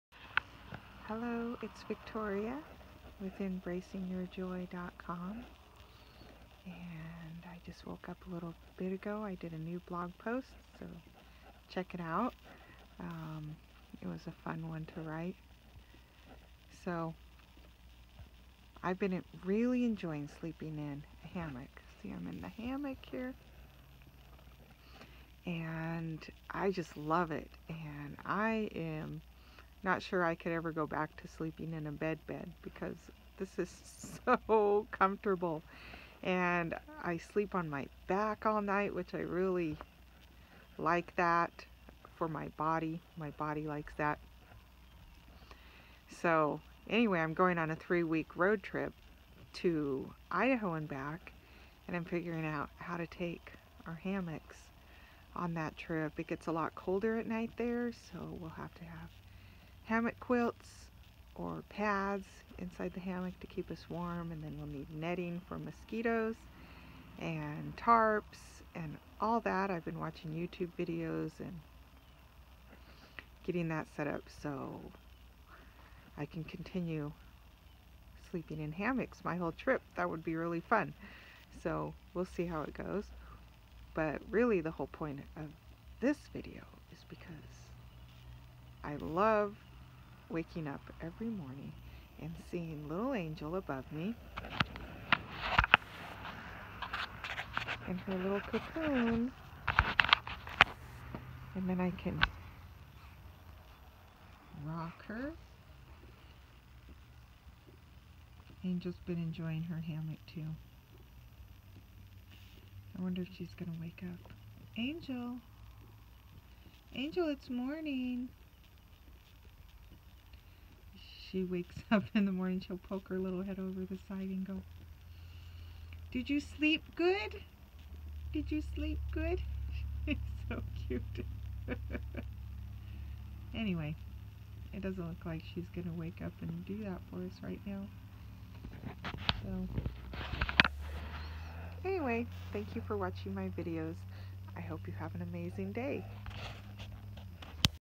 I love love love sleeping in a hammock. My 3 yea old does too!
Talking about sleeping in a hammock.